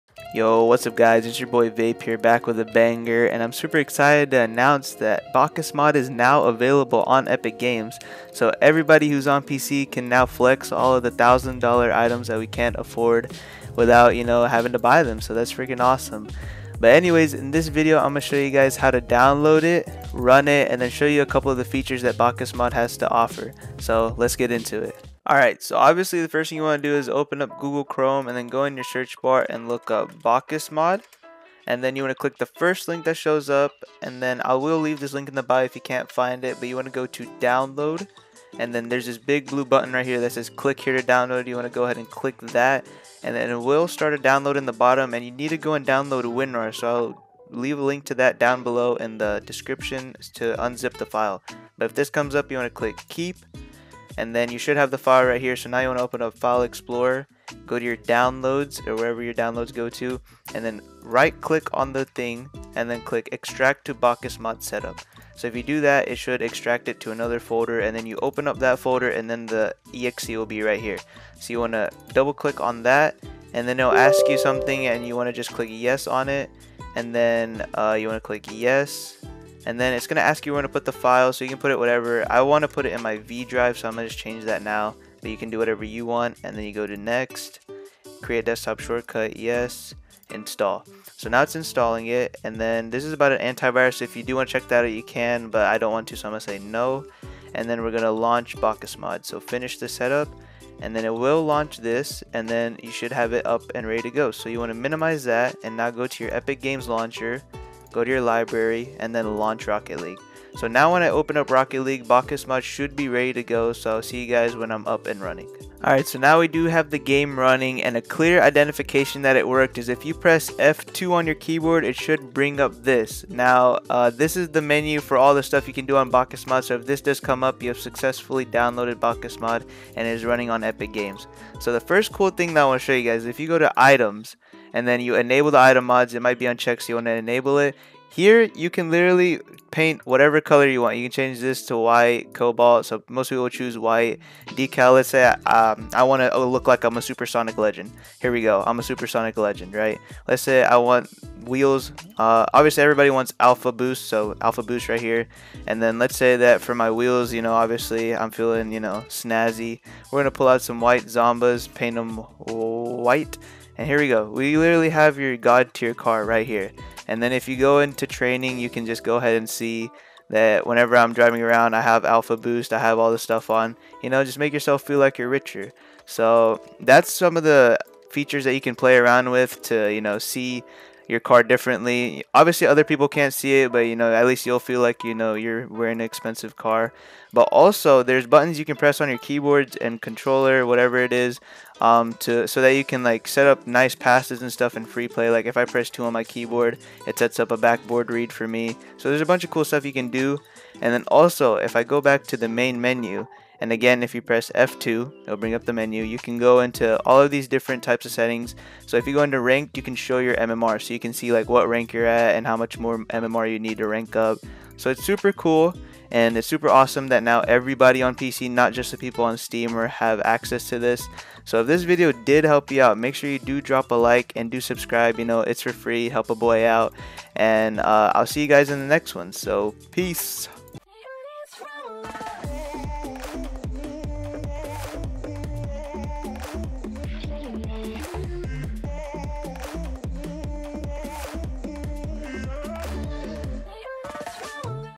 How To Download And Use BakkesMod On Epic Games! *(2021)*
Here's a guide on how to download BakkesMod for Epic Games!

Stalk me on Twitch!

Like the music on my channel? Here's a Spotify playlist of some of my favorites